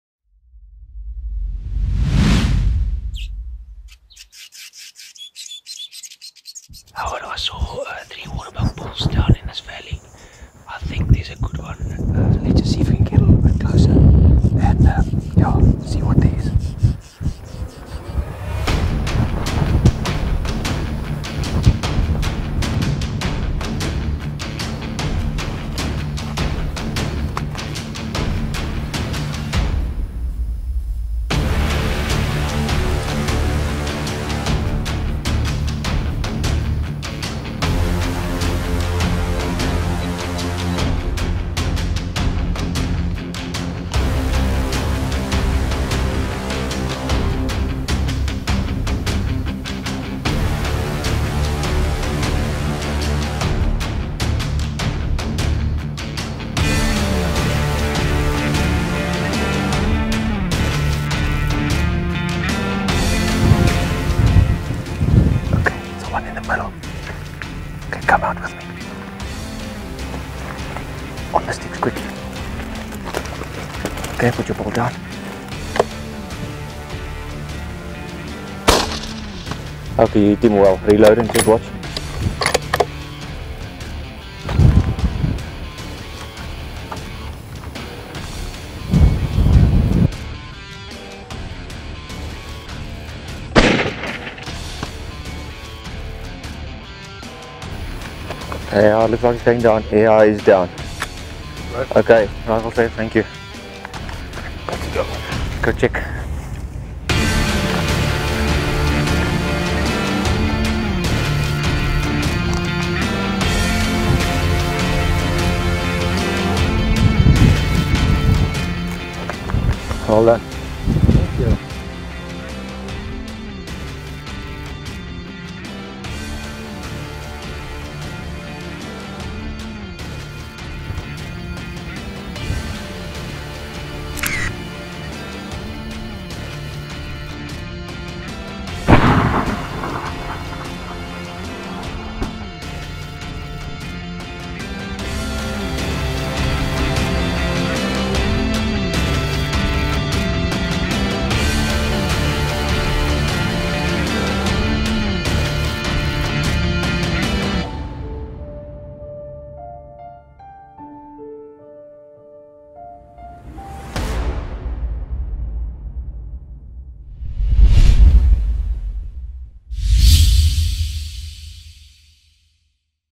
Waterbuck Hunting
When Howard visits us from the United States, we hunt long and hard. This time was no different.
We spotted a small group of Waterbuck bulls and one bull in particular looked like the perfect animal to go after.